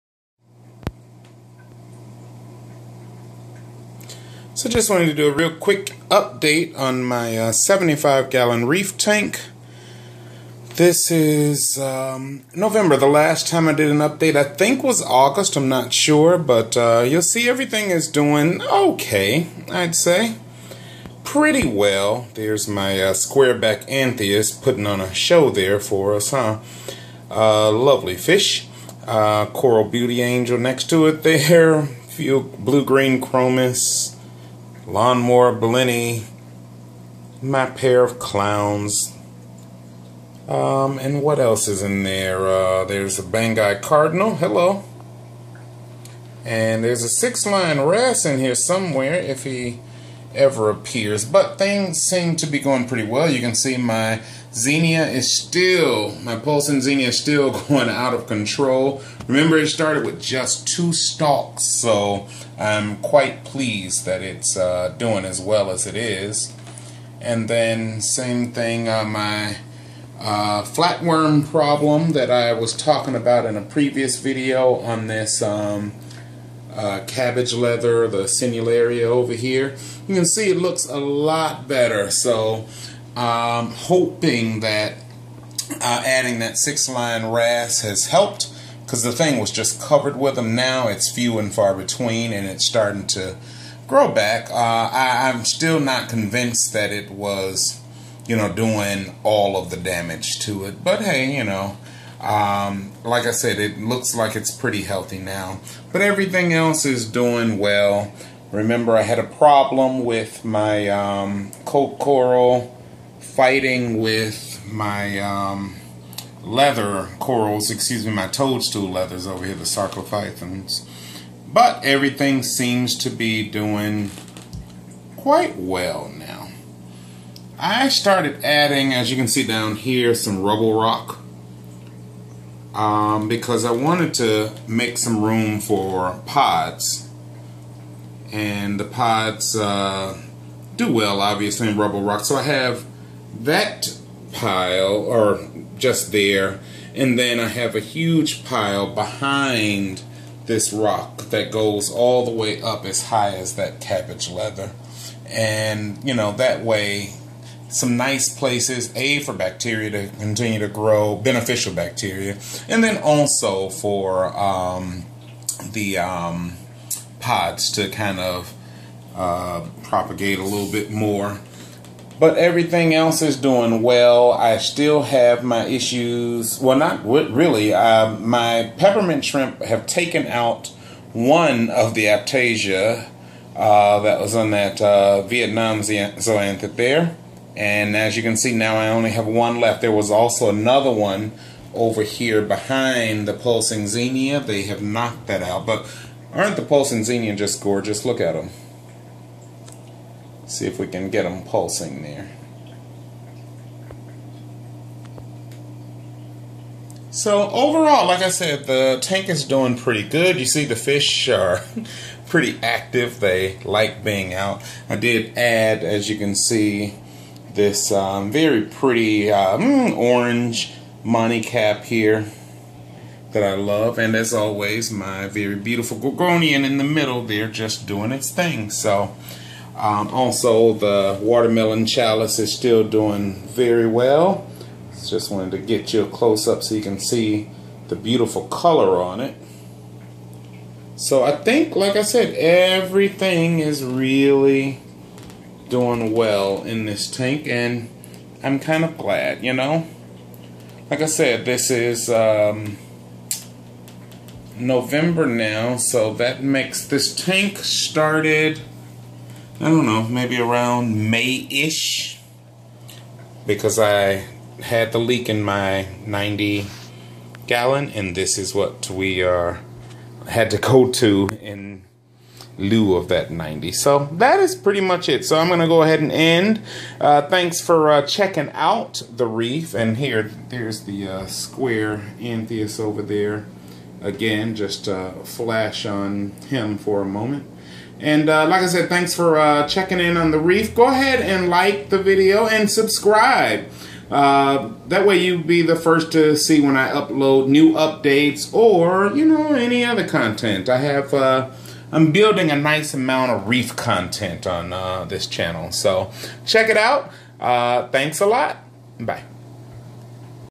75 Gallon Reef - 6 Month Update
75 gallon reef update after 6 months.
♥ Follow Jimmy Lee Lard – The Sock Snob ♥

or The Cooter Brownlee Series to find out who killed Tupac.